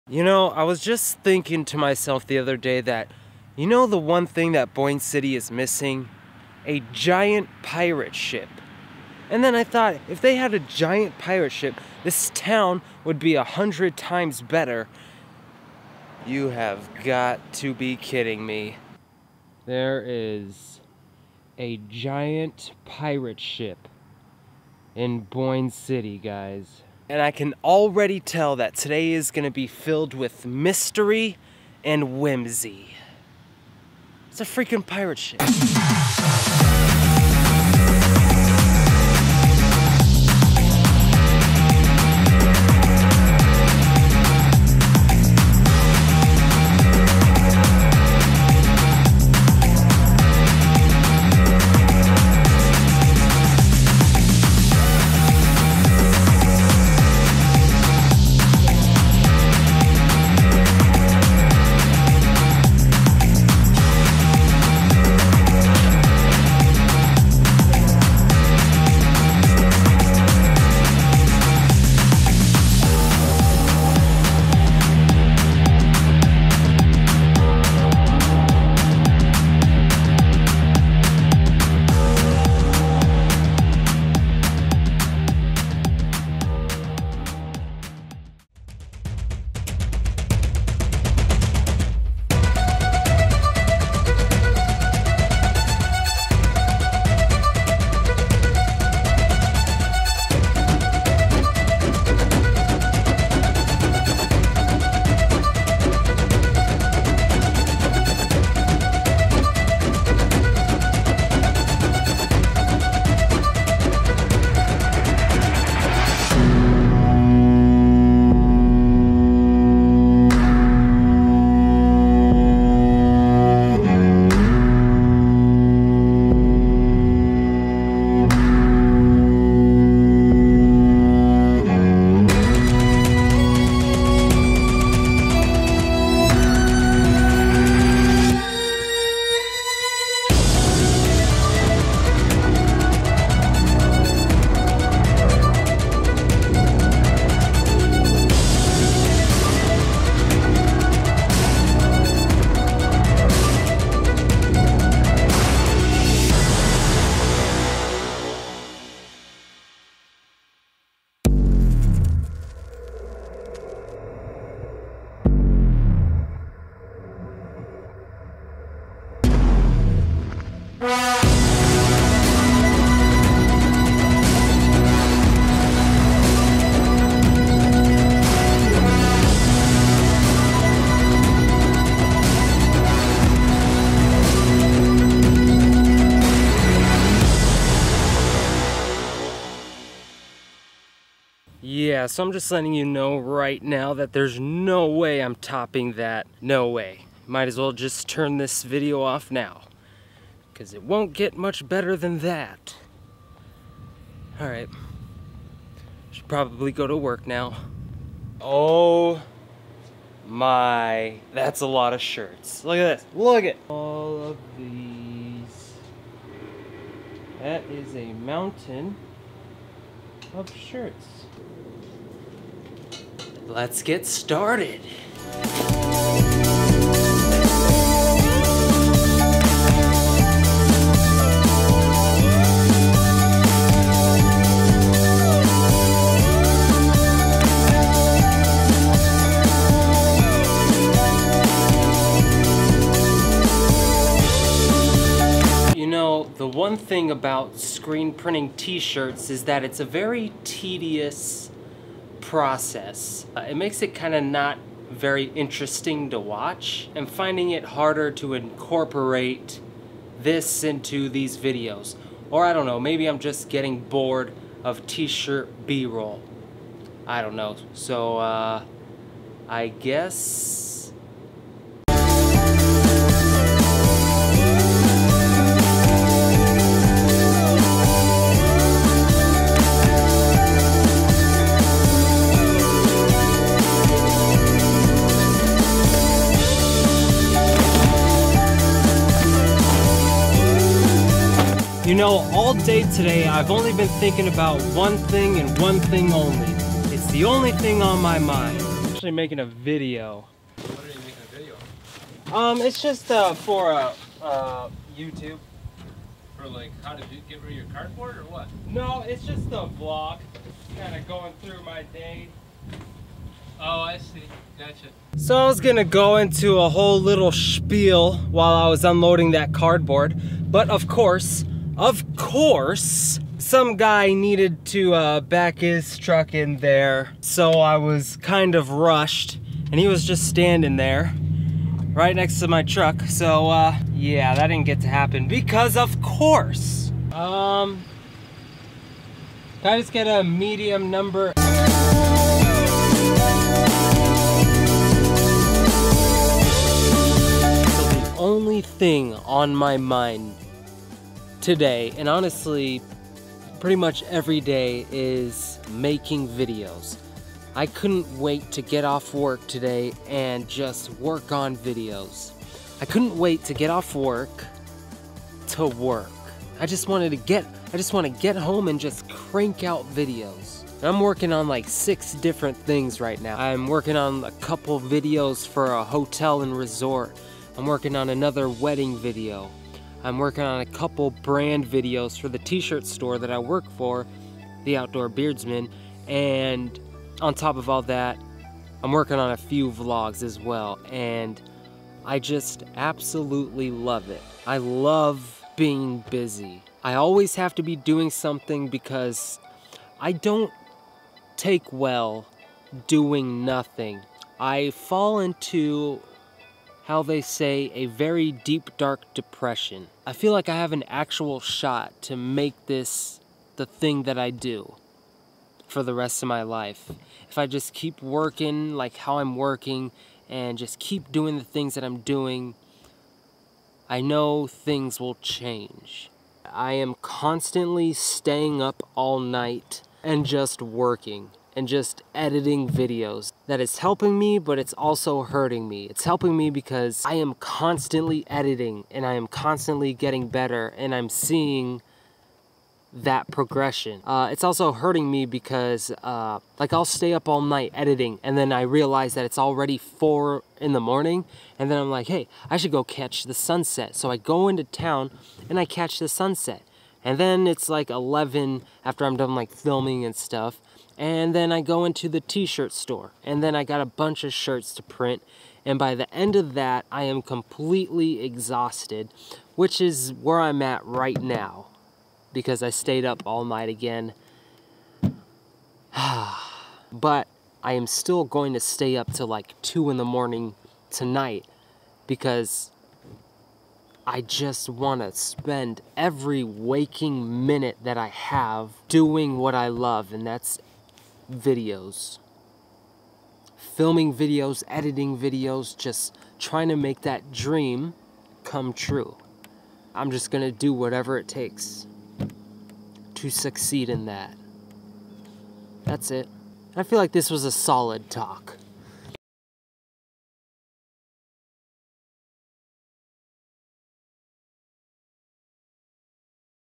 GIANT PIRATE SHIP IN BOYNE CITY!!
There's a giant pirate ship in boyne city I decided to check out. The b roll from this video got me a free ride on this giant ship too! (That video is coming next)